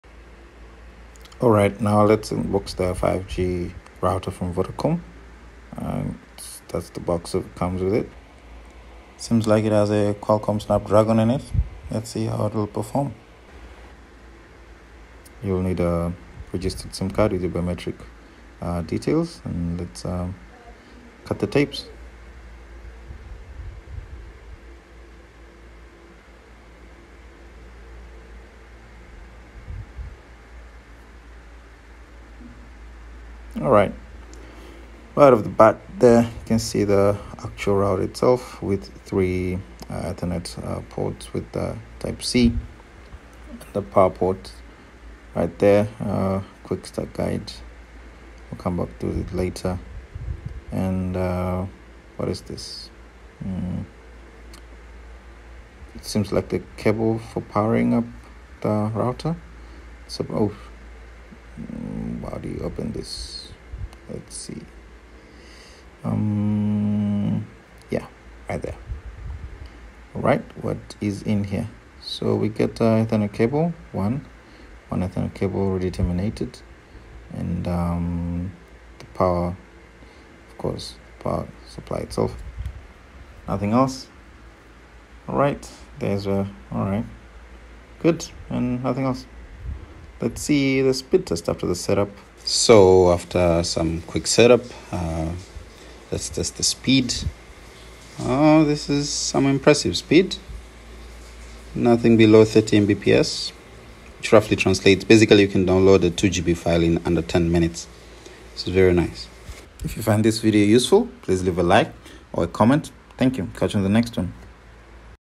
Unboxing Vodacom 5G Router
The unboxing experience and speed test of Vodacom 5G router.